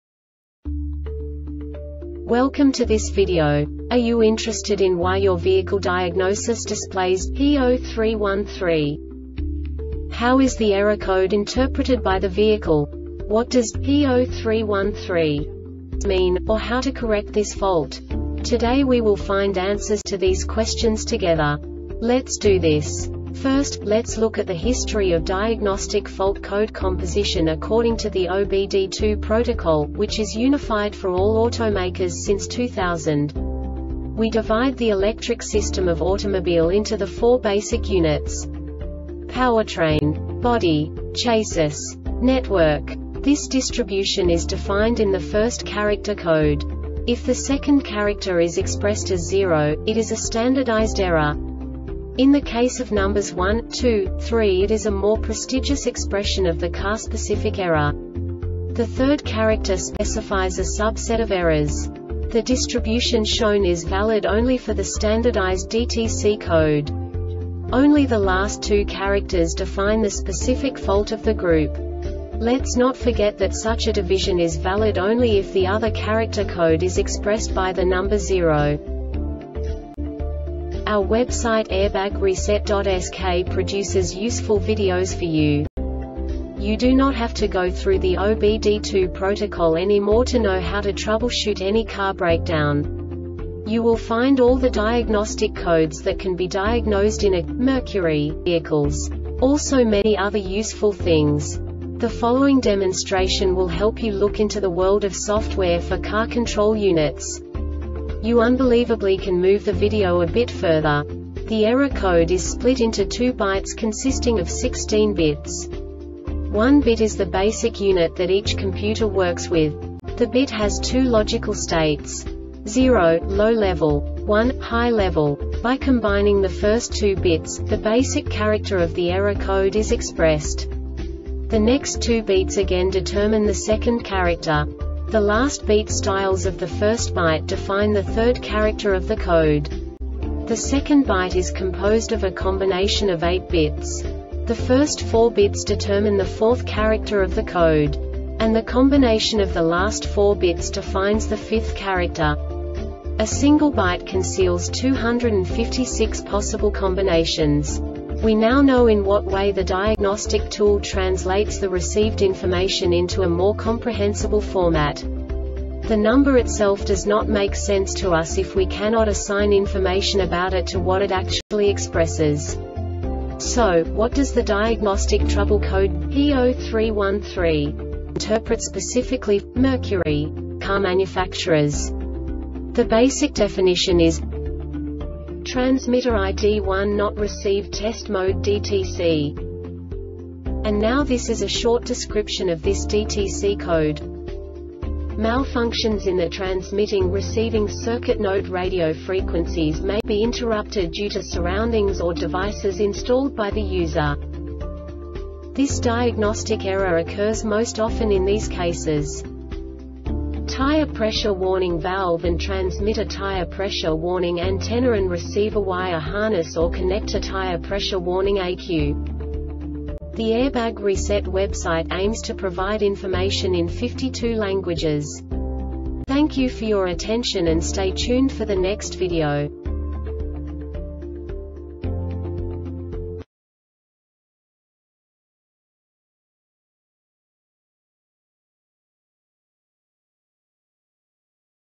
DTC Mercury P0313 Short Explanation
Contents:
0:21 Basic DTC analysis according to OBD2 protocol standard.
1:48 Insight into programming
2:58 A diagnostically understandable description of the Diagnostic Trouble Code
3:05 Basic error definition

Make
Mercury
Code
P0313
Definition
Transmitter ID1 not Received Test Mode DTC
Description
Malfunctions in the transmitting or receiving circuit  NOTE Radio frequencies may be interrupted due to surroundings or devices installed by the user
Cause
 Tire pressure warning valve and transmitter  Tire pressure warning antenna and receiver  Wire harness or connector  Tire pressure warning ECU